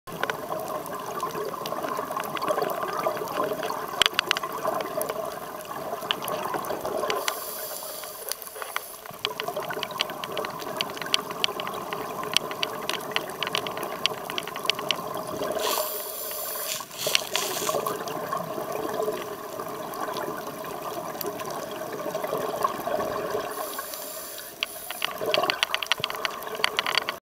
Grey reef sharks in line up
Grey reefsharks at Peleliu corner, Palau in november 2008.